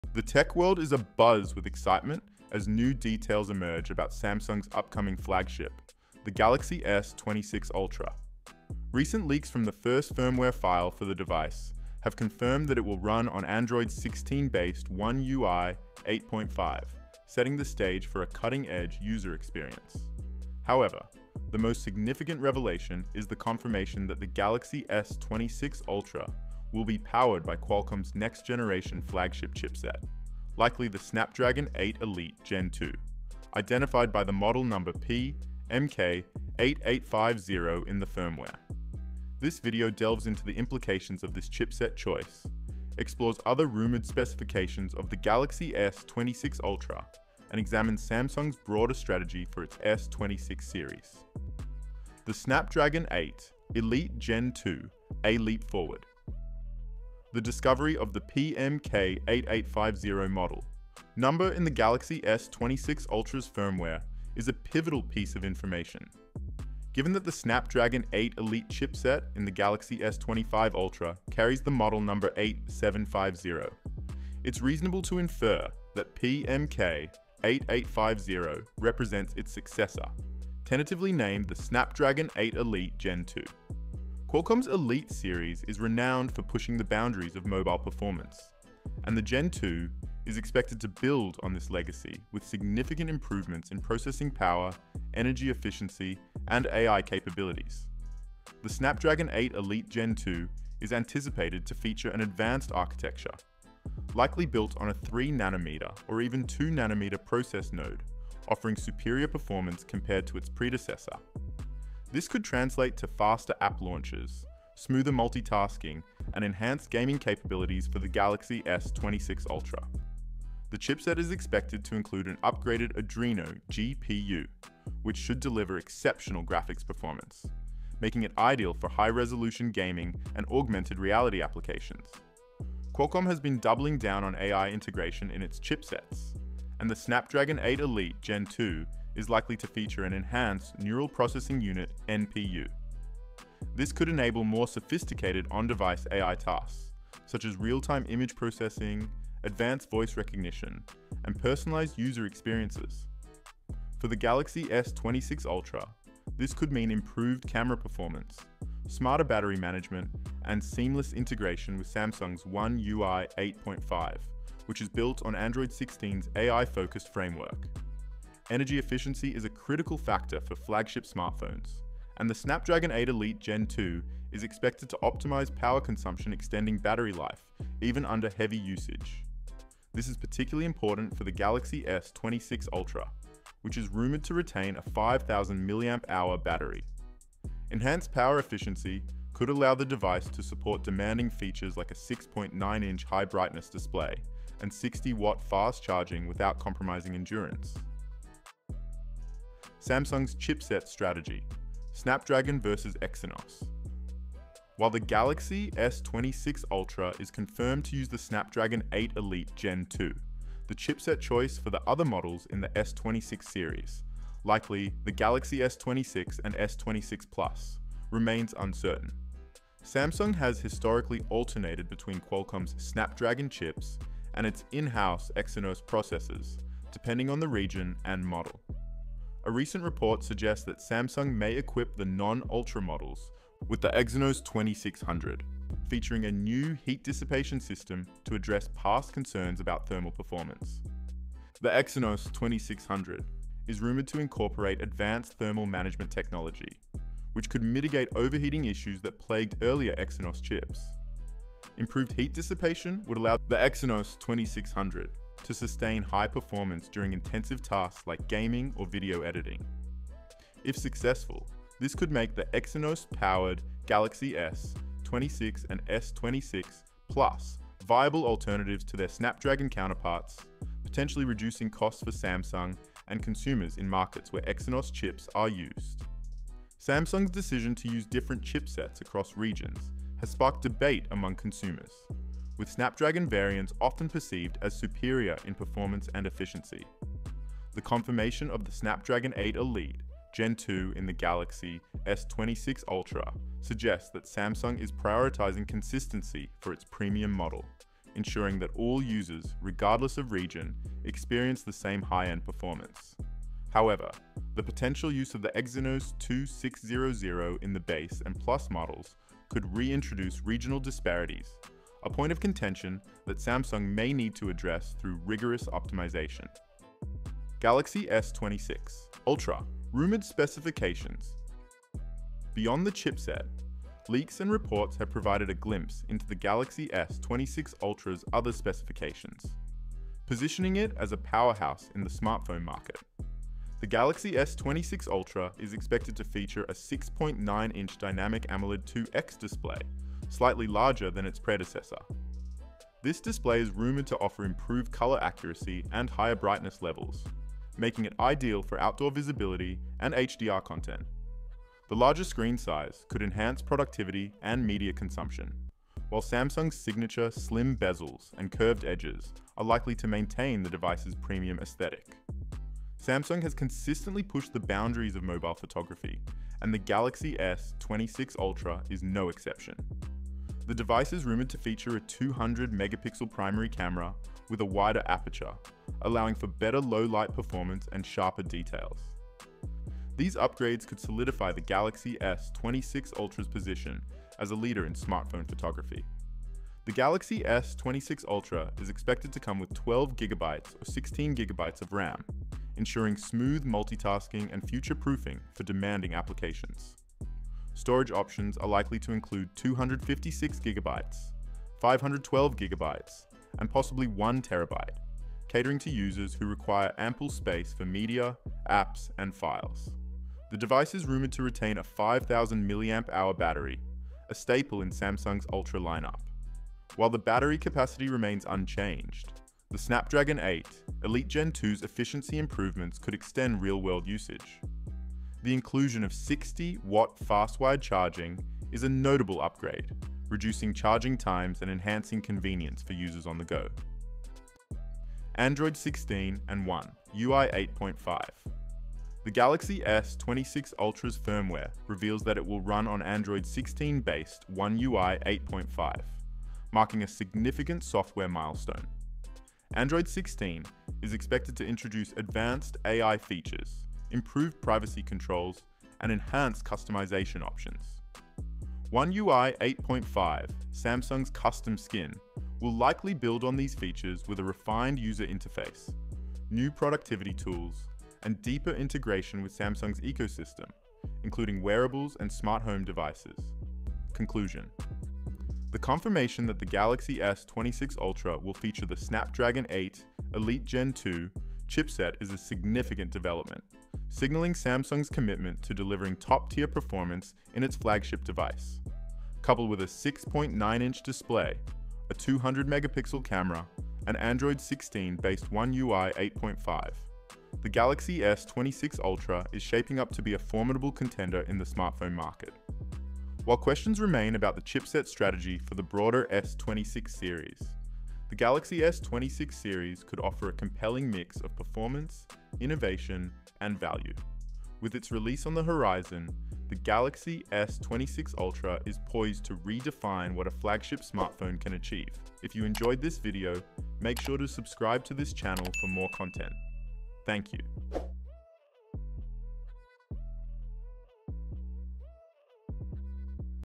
Galaxy S26 Ultra CONFIRMED with Snapdragon 8 Elite Gen 2 – Specs, Features & Leaks
It’s official — the Samsung Galaxy S26 Ultra will feature Qualcomm’s next-gen Snapdragon 8 Elite Gen 2 chipset, promising unmatched performance, AI power, and battery efficiency.

In this video, we break down the confirmed details, rumored specs, and key upgrades — including a massive 6.9-inch Dynamic AMOLED 2X display, 200MP camera, Android 16-based One UI 8.5, and blazing-fast 60W charging.

We also explore Samsung’s chipset strategy for the S26 series and how the Ultra stacks up against competitors like the iPhone 18 Pro Max and Pixel 11 Pro.